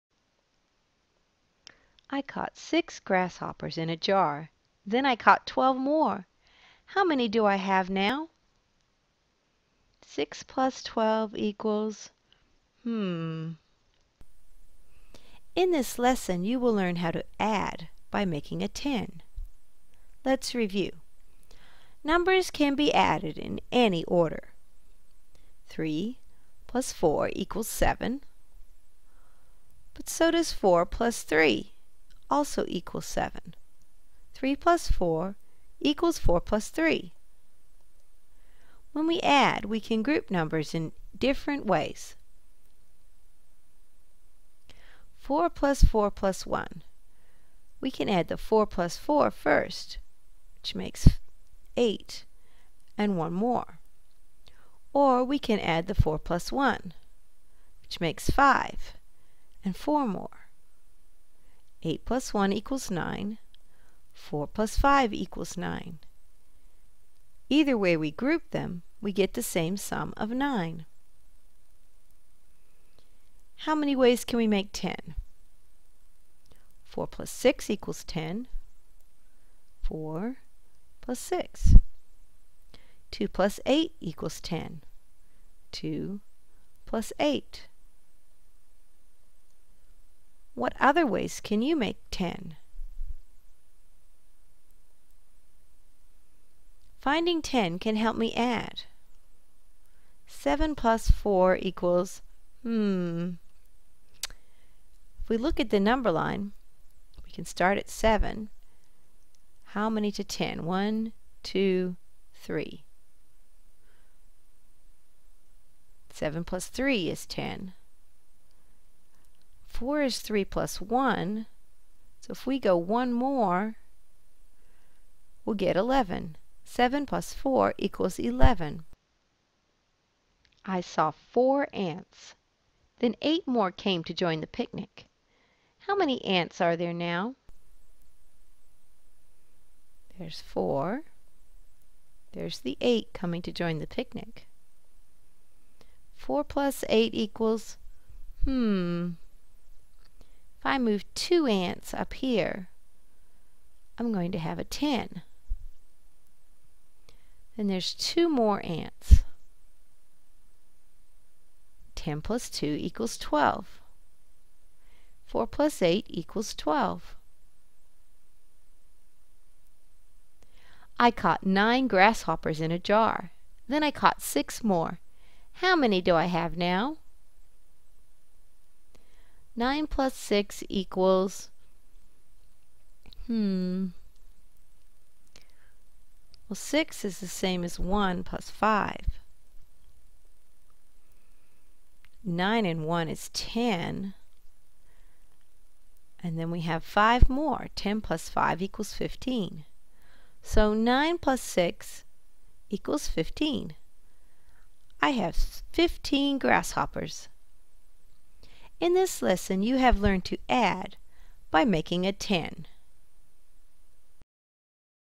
Add by making a ten
In this lesson you will learn to add by making a ten.

ADDITIONAL MATERIALS

STANDARDS

CCSS.2.OA.B.2 Fluently add and subtract within 20 using mental strategies. By end of Grade 2, know from memory all sums of two one-digit numbers. See standard 1.OA.6 for a list of mental strategies.

TEKS.2.4.A recall basic facts to add and subtract within 20 with automaticity;

TEKS.1.3.D apply basic fact strategies to add and subtract within 20, including making 10 and decomposing a number leading to a 10;

VA.CE.2.6.b determine sums and differences, using various methods; and

VA.CE.2.5.b demonstrate fluency with addition and subtraction within 20.

FL.MAFS.2.OA.2.2 Fluently add and subtract within 20 using mental strategies. By end of Grade 2, know from memory all sums of two one-digit numbers.